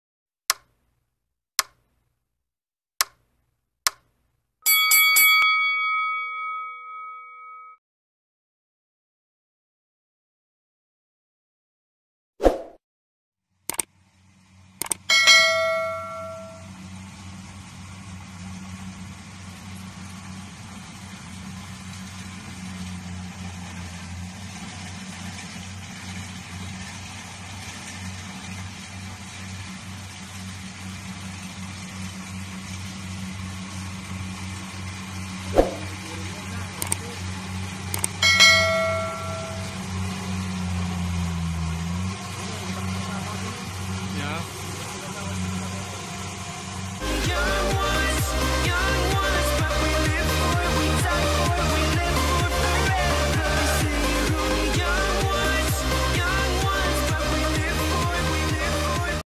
HARVESTING TIME/LULUH CHANNEL
Publish on October 5,2020

It's harvesting time😊😊😊🙏🙏
Just sharing you this short video guys on how they harvest rice plants in our place